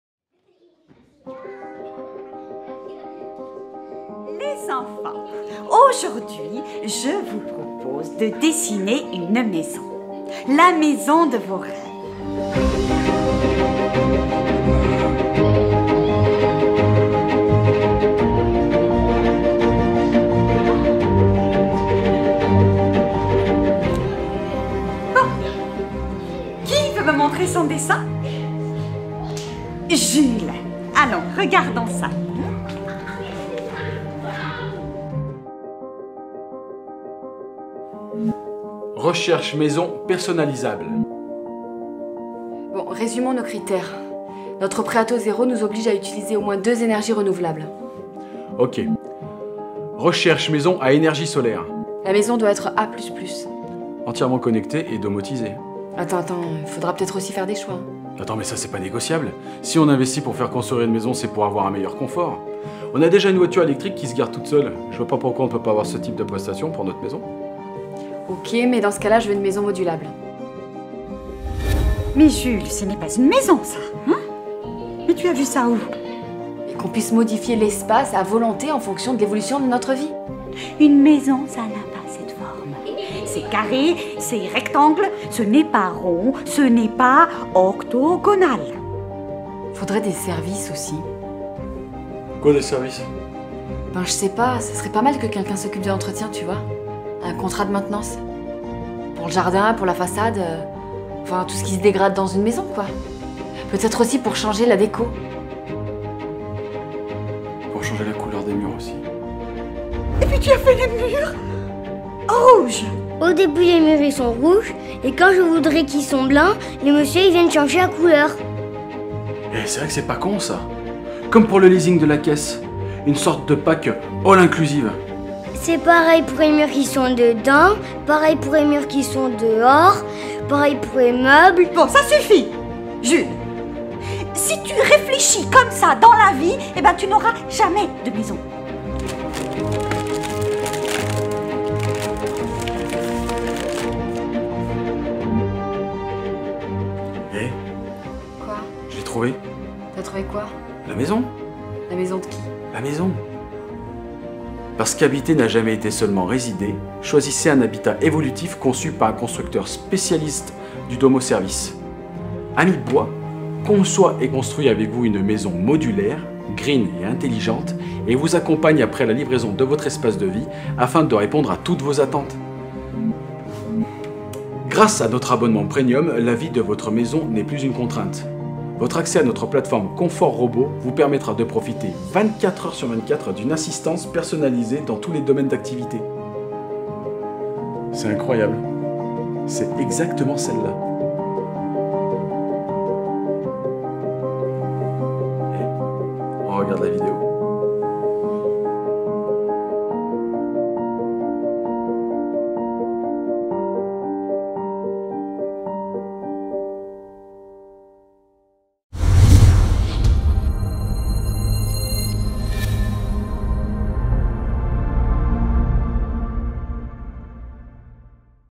Ami Bois - Concept House, médaille d'argent UMF 2016
Soutenu par ce film publicitaire, Ami Bois a remporté la médaille d'argent à l'UMF 2016 dans la catégorie "Concept House".
Un film Digital Gate
digitalgate.fr